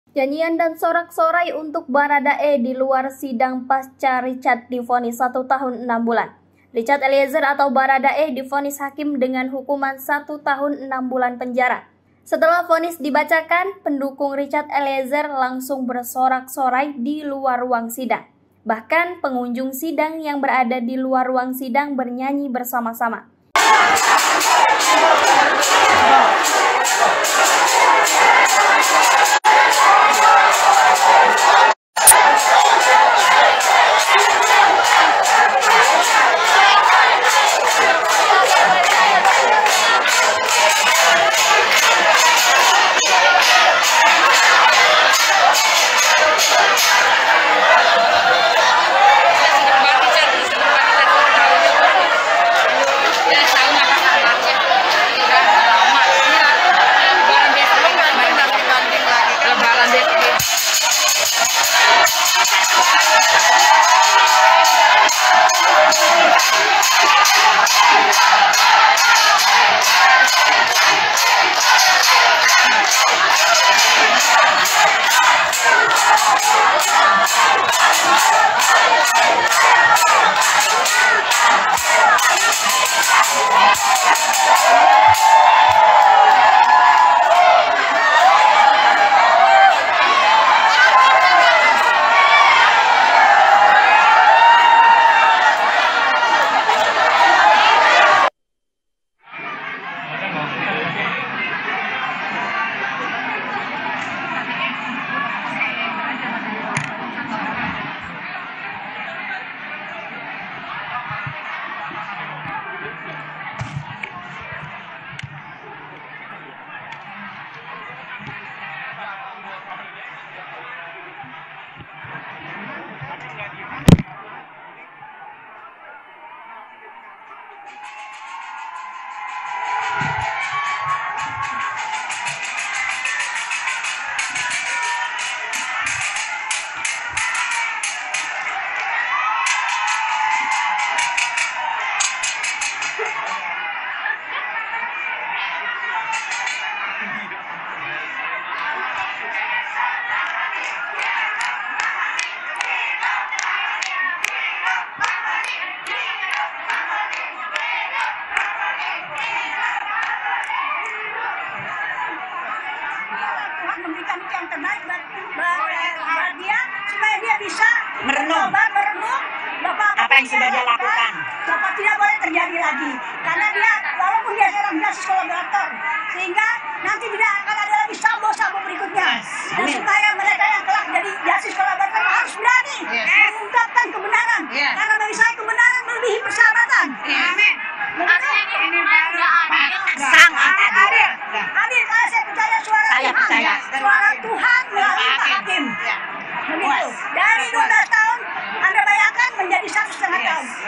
Nyanyian dan Sorak Sorai untuk Bharada E di Luar Ruang Sidang Pasca Richard Divonis 1 Tahun 6 Bulan
TRIBUNBENGKULU.COM - Richard Eliezer atau Bharada E divonis hakim dengan hukuman satu tahun enam bulan penjara.

Setelah vonis dibacakan, pendukung Richard Eliezer langsung bersorak sorai di luar ruang sidang.

Bahkan, pengunjung sidang yang berada di luar ruang sidang bernyanyi bersama-sama.

Sidang digelar hari ini sodara, Rabu (15/2/2023) di PN Jaksel.